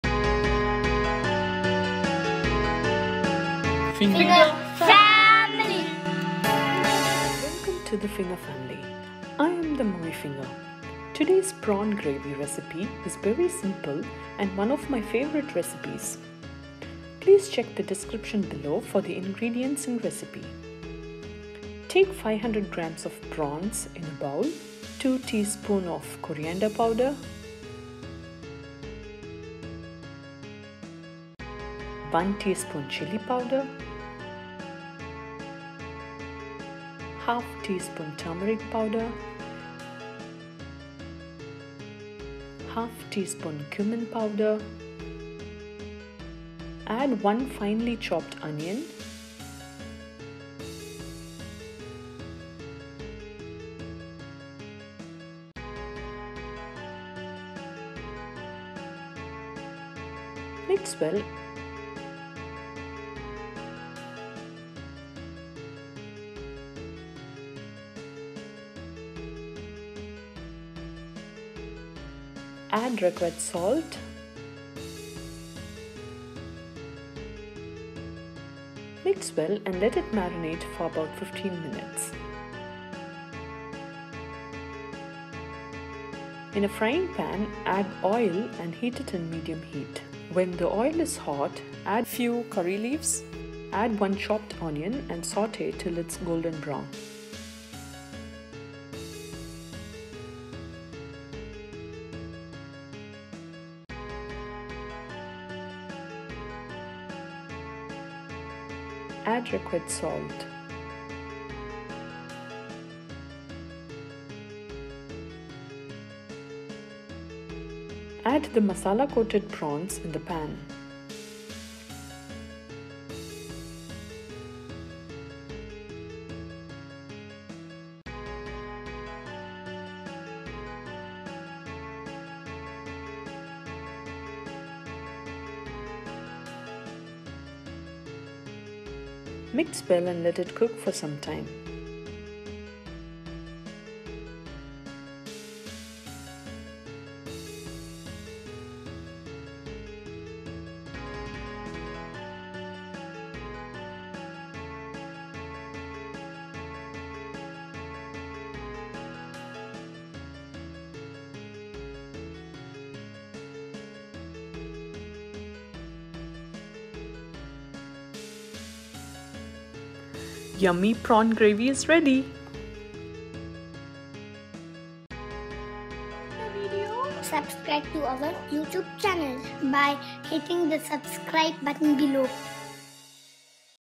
Prawn Gravy (🖐 Finger Family Cooking E13)
Ingredients:
- 500g prawns.
- 2 tsp coriander powder
- 1 tsp chili powder
- 1/2 tsp turmeric powder
- 1/2 tsp cumin powder
- 1 onion
- salt
- Oil
- 1 onion

Few useful Finger Family links:
(like the Page for live updates)

Today’s prawn gravy recipe is very simple and one of my favourite recipes.

Take 500g prawns in a bowl.
Add 2 tsp coriander powder
1 tsp chili powder
1/2 tsp turmeric powder
1/2 tsp cumin powder
1 finely chopped onion
Mix well
Add required salt
Mix well and I let it marinate for 15 minutes.

In a frying pan, add oil and heat it in medium heat.
When the oil is hot,  add few curry leaves , Add 1 chopped onion and sauté till it’s golden brown.
Add required salt.

Add the masala coated prawns in the pan. Mix well and let it cook for sometime.

Yummy prawn fry is ready!